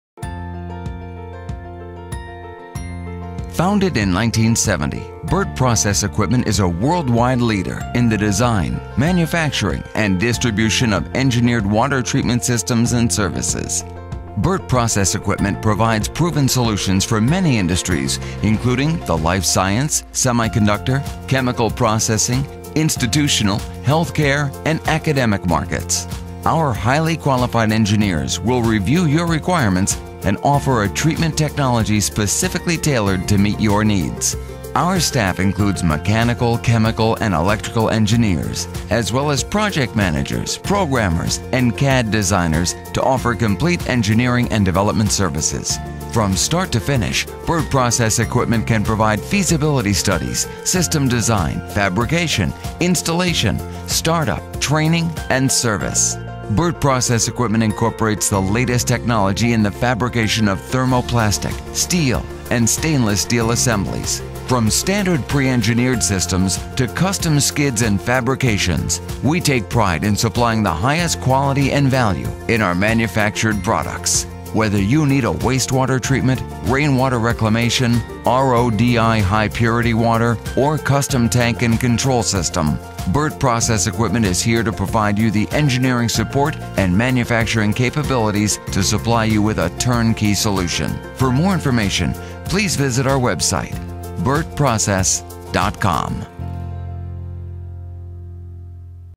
Burt Process Equipment Manufactured Systems
Founded in 1970 Burt Process Equipment is a worldwide leader in the design, manufacturing, and distribution of high purity and corrosion-resistant equipment, systems, and services. BPE products and services provide proven solutions for many industries, including pharmaceutical, biotechnology, microelectronics, metal finishing, and chemical processing.